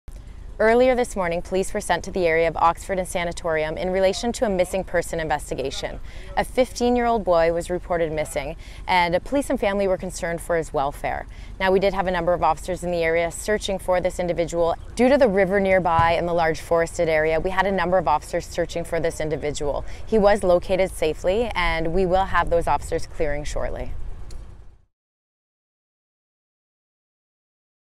Police find missing teen in West London after short search
A 15-year-old boy was found in the Sanatorium and Oxford area after a short search this morning.